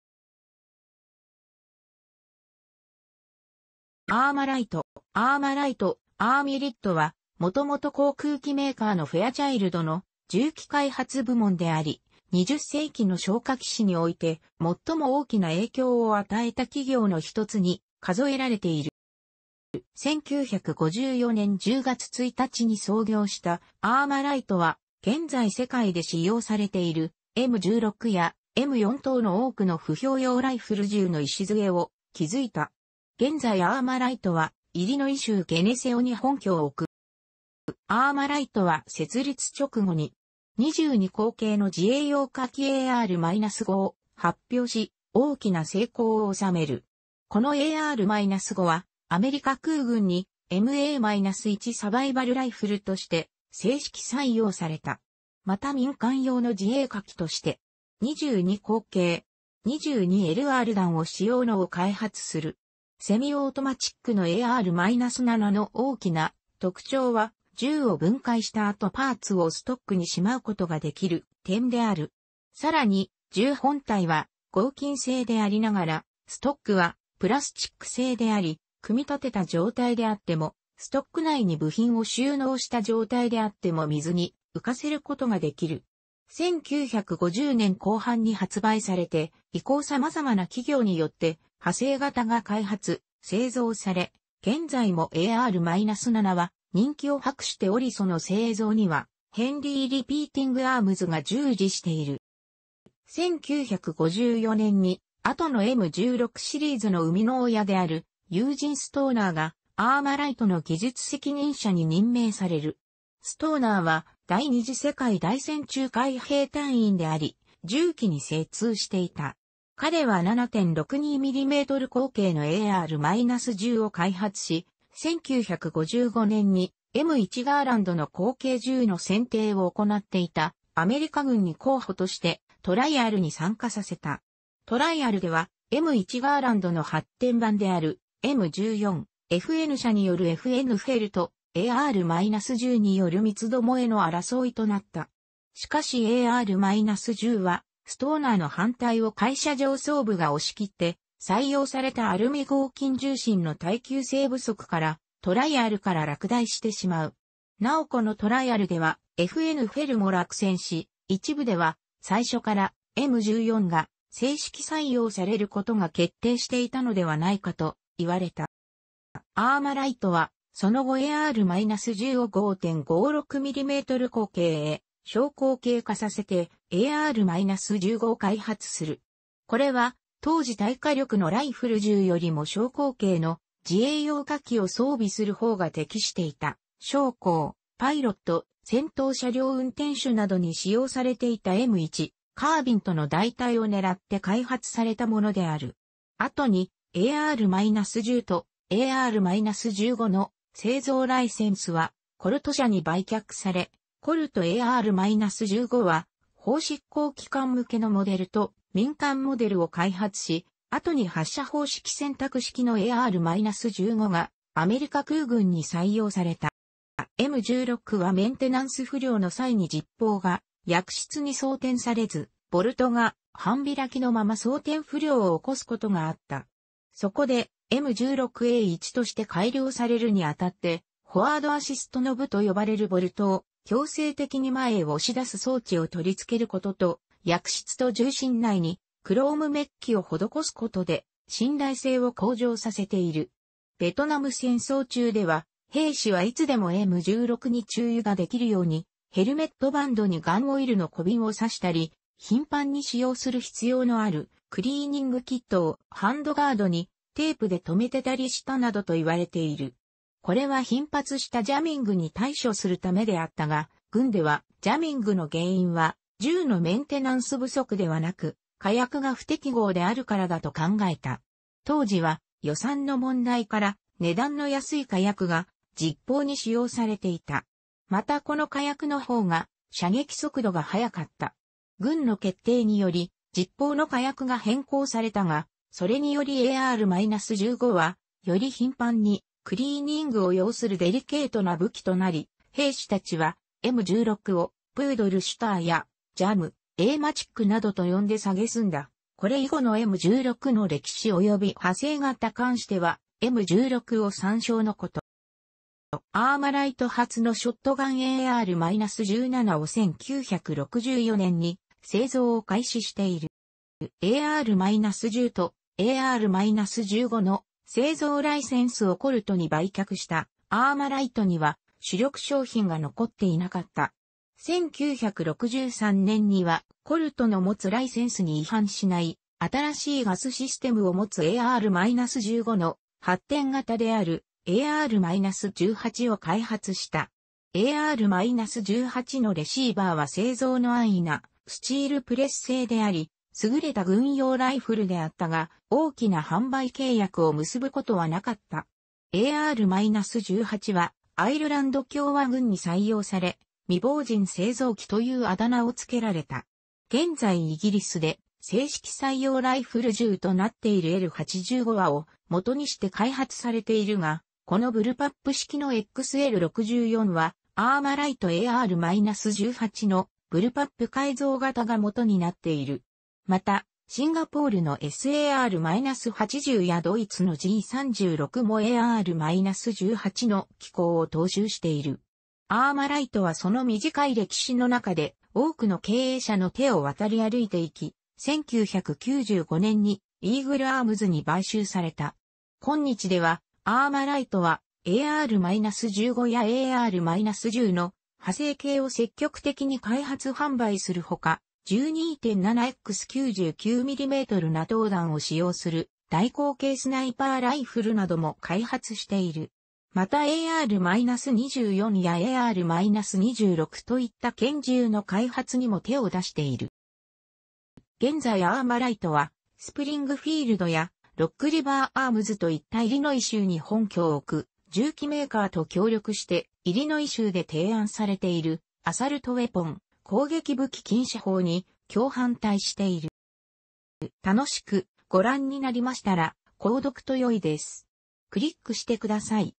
アーマライト
アーマライト（ArmaLite）は、元々航空機メーカーのフェアチャイルドの銃器開発部門であり、20世紀の小火器史において最も大きな影響を与えた企業の一つに数えられている。

1954年10月1日に創業したアーマライトは現在世界で使用されているM16やM4等の多くの歩兵用ライフル銃の礎を築いた。現在アーマライトはイリノイ州ゲネセオに本拠を置く。

アーマライトは設立直後に.22口径の自衛用火器AR-5を発表し、大きな成功を収める。このAR-5はアメリカ空軍にMA-1サバイバルライフルとして制式採用された。また民間用の自衛火器として.22口径（.22LR弾を使用）のを開発する。セミオートマチックのAR-7の大きな特徴は、銃を分解した後パーツをストックにしまうことができる点である。更に銃本体は合金製でありながら、ストックはプラスチック製であり、組み立てた状態であっても、ストック内に部品を収納した状態であっても水に浮かせることができる。1950年後半に発売されて以降様々な企業によって派生型が開発・製造され、現在もAR-7は人気を博しておりその製造にはヘンリー・リピーティングアームズが従事している。
1954年に後のM16シリーズの生みの親であるユージン・ストーナーがアーマライトの技術責任者に任命される。ストーナーは第二次世界大戦中海兵隊員であり、銃器に精通していた。彼は7.62mm口径のAR-10を開発し、1955年にM1ガーランドの後継銃の選定を行っていたアメリカ軍に候補としてトライアルに参加させた。トライアルではM1ガーランドの発展版であるM14、FN社によるFN FALとAR-10による三つ巴の争いとなった。しかしAR-10はストーナーの反対を会社上層部が押し切って採用されたアルミ合金製銃身の耐久性不足からトライアルから落第してしまう。なおこのトライアルではFN FALも落選し、一部では最初からM14が制式採用されることが決定していたのではないかと言われた。

アーマライトはその後AR-10を5.56mm口径へ小口径化させてAR-15を開発する。これは当時大火力のライフル銃よりも小口径の自衛用火器を装備する方が適していた将校、パイロット、戦闘車両運転手などに使用されていたM1カービンとの代替を狙って開発されたものである。後にAR-10とAR-15の製造ライセンスはコルト社に売却され、コルトAR-15は法執行機関向けのモデルと民間モデルを開発し、後に発射方式選択式のAR-15がアメリカ空軍に採用された。

M16はメンテナンス不良の際に実包が薬室に装填されず、ボルトが半開きのまま装填不良を起こすことがあった。そこでM16A1として改良されるにあたって、フォアードアシストノブと呼ばれるボルトを強制的に前へ押し出す装置を取り付けることと、薬室と銃身内にクロームメッキをほどこすことで、信頼性を向上させている。ベトナム戦争中では兵士はいつでもM16に注油ができるようにヘルメットバンドにガンオイルの小瓶を挿したり、頻繁に使用する必要のあるクリーニングキットをハンドガードにテープで留めていたりしたなどと言われている。これは頻発したジャミングに対処するためであったが、軍ではジャミングの原因は銃のメンテナンス不足ではなく、火薬が不適合であるからだと考えた。当時は予算の問題から値段の安い火薬が実包に使用されていた。またこの火薬の方が、射撃速度が速かった。軍の決定により実包の火薬が変更されたが、それによりAR-15はより頻繁にクリーニングを要するデリケートな武器となり、兵士達はM16を「Poodle Shooter」や「Jam A Matic」などと呼んで蔑んだ（これ以後のM16の歴史及び派生型に関してはM16を参照のこと）。

アーマライト初のショットガンAR-17を1964年に製造を開始している。

AR-10とAR-15の製造ライセンスをコルトに売却したアーマライトには主力商品が残っていなかった。1963年にはコルトの持つライセンスに違反しない、新しいガスシステムを持つAR-15の"発展型"であるAR-18を開発した。AR-18のレシーバーは製造の安易なスチールプレス製であり、優れた軍用ライフルであったが、大きな販売契約を結ぶことはなかった。AR-18はアイルランド共和軍 (IRA) に採用され、「未亡人製造機 (widowmaker)」という...